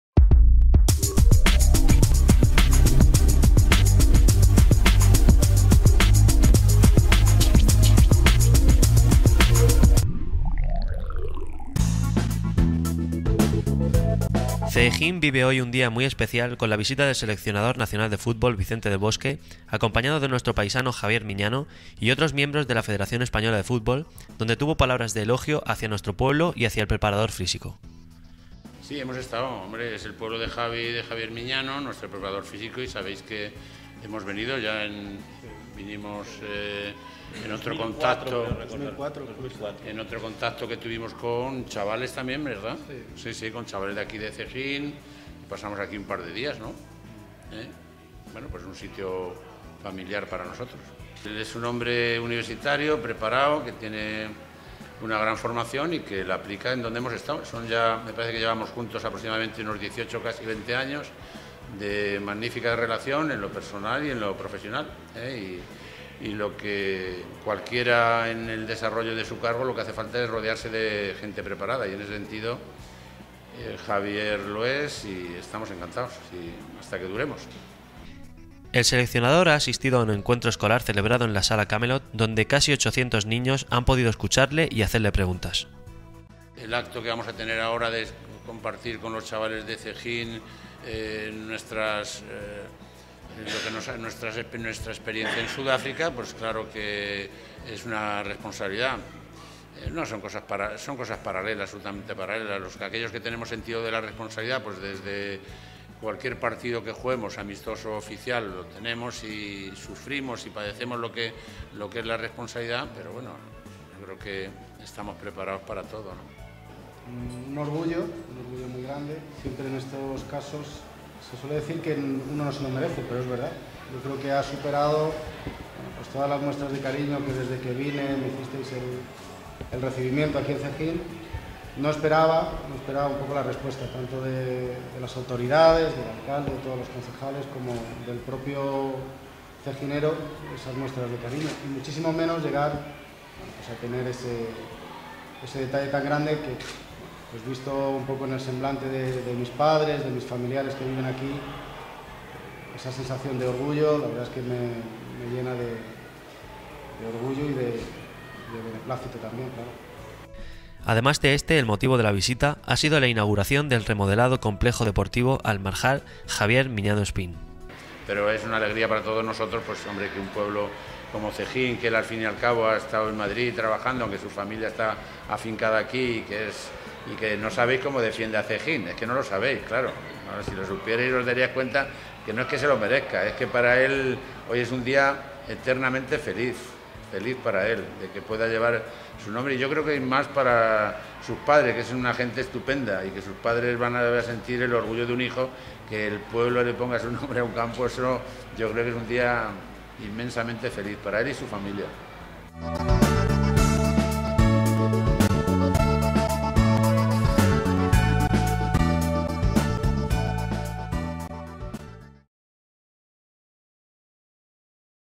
Encuentro escolar en Cehegín con Vicente del Bosque
Los niños de Cehegín han podido disfrutar de Vicente del Bosque, Javier Miñano y otros miembros de la Federacion Española de Futbol en la sala Camelot. El motivo de su visita es, además, la inauguración del remodelado Polideportivo Almarjal "Javier Miñano Espín".